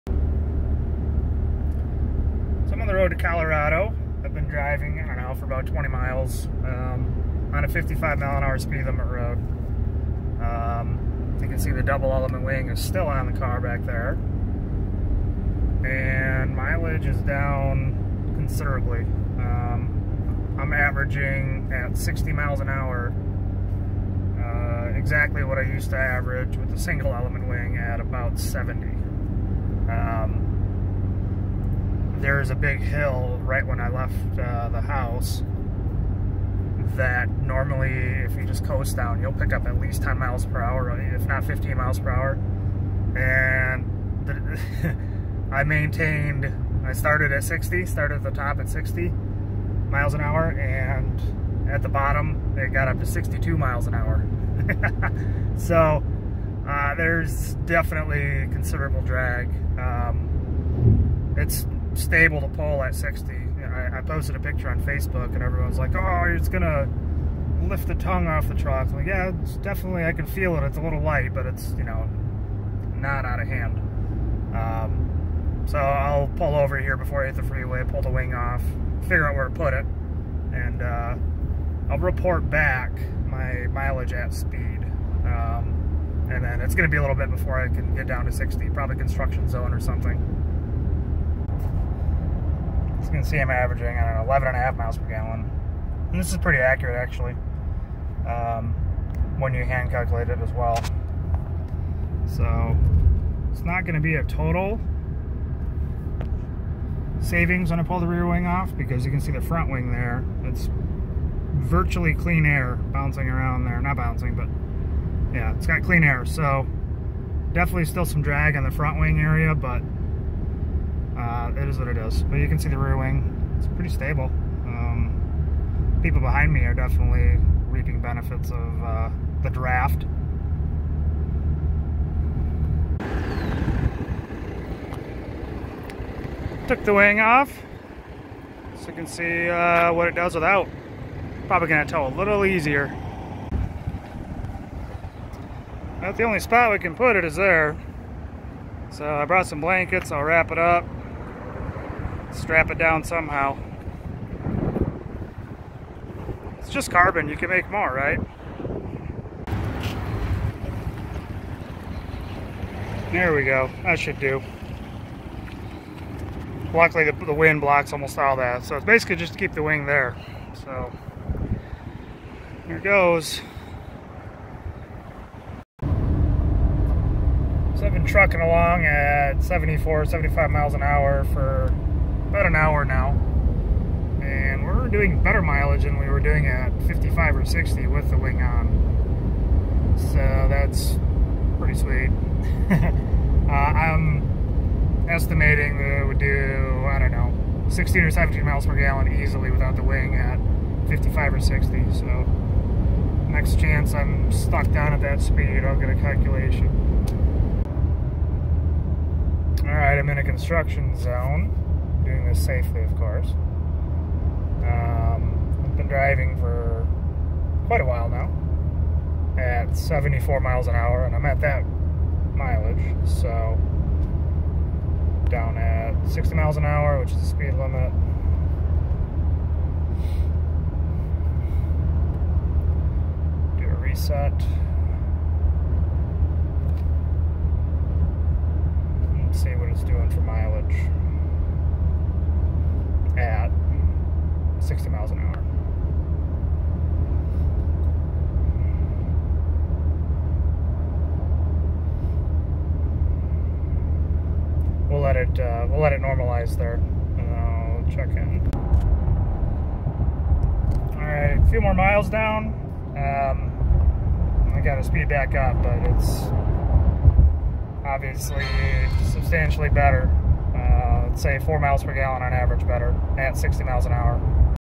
Beasties huge wing and towing MPG
I run some semi-scientific towing experiments on the way to Pikes peak